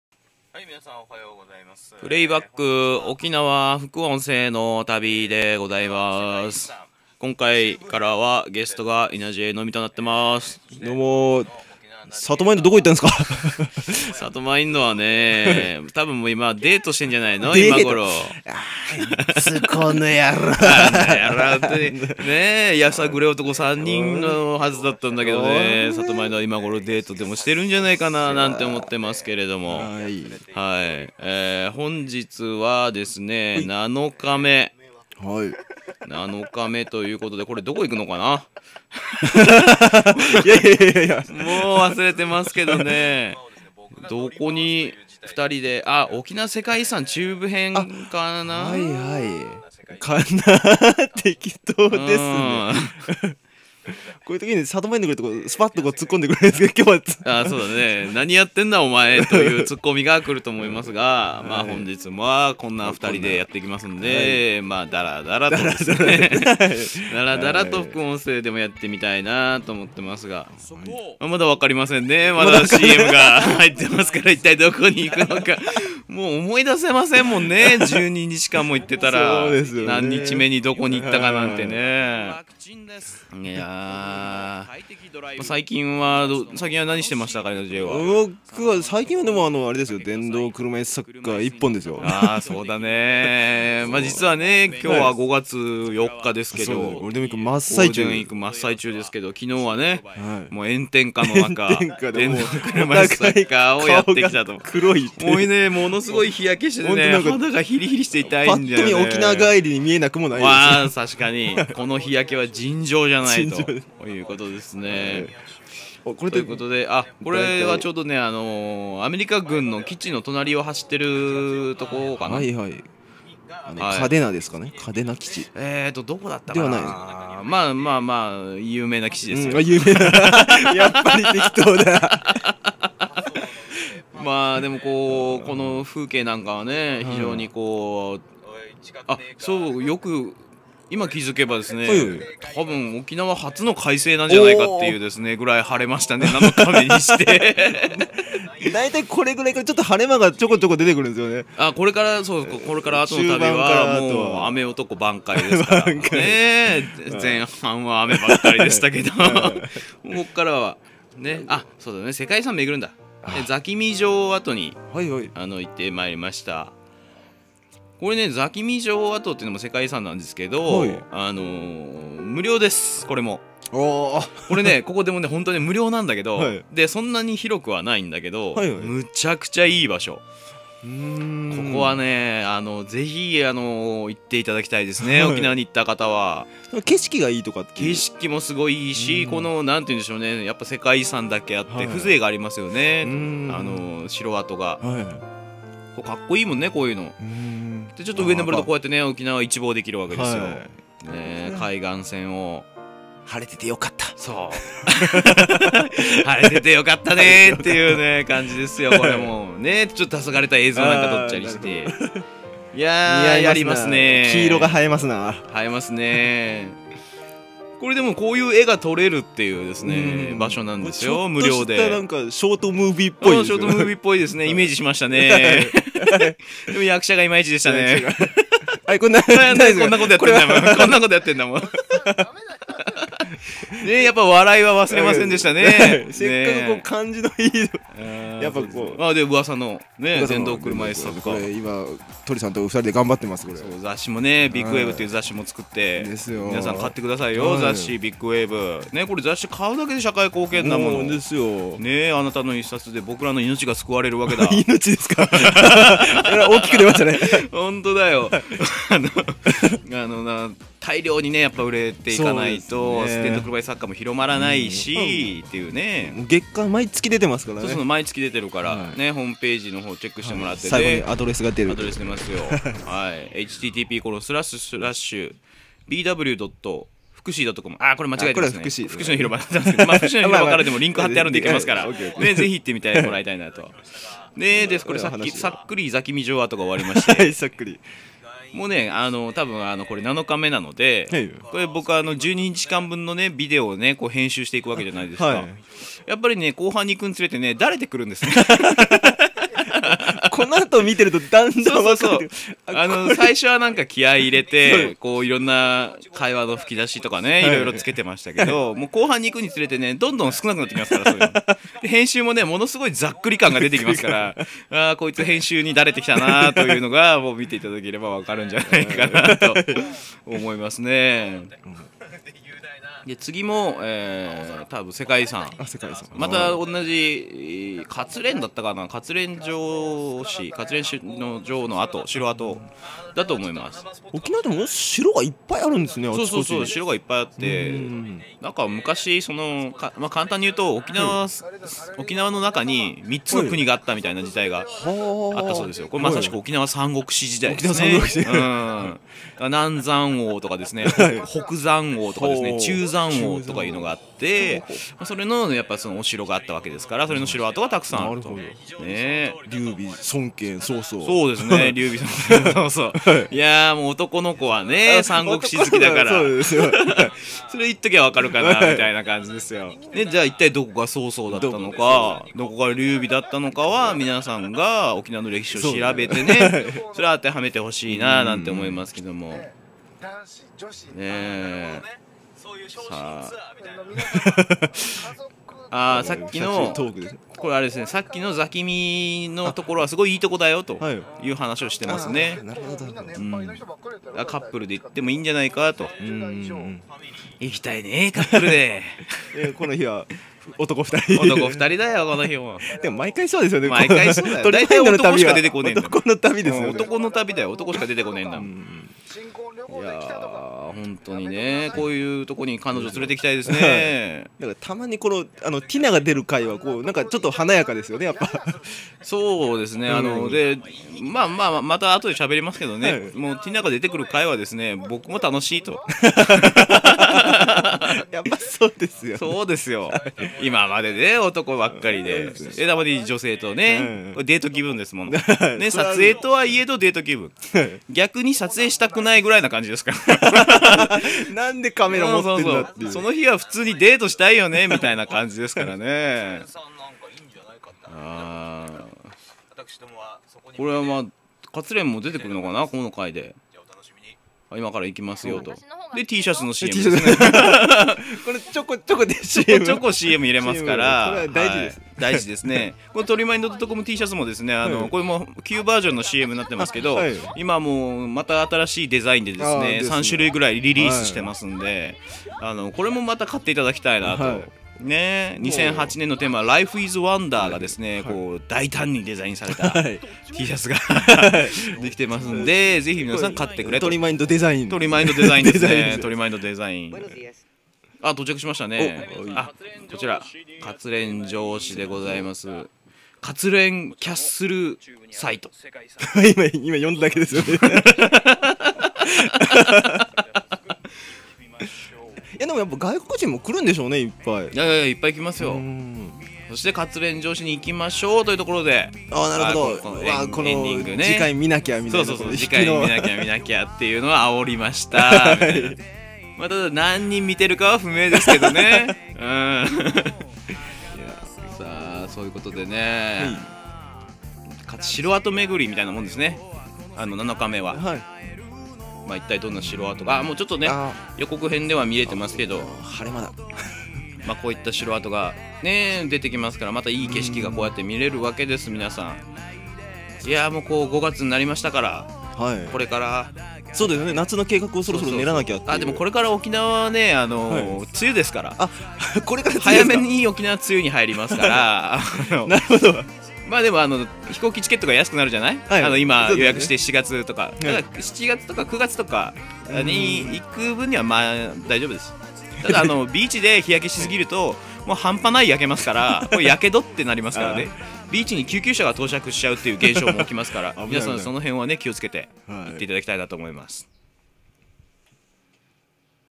僕の沖縄12日間 第7話 副音声 前編
TORIMIND.COM 2007

南国のパラダイス沖縄。観光地をほぼ網羅する12日間！台風も呼んじゃった旅をお楽しみください！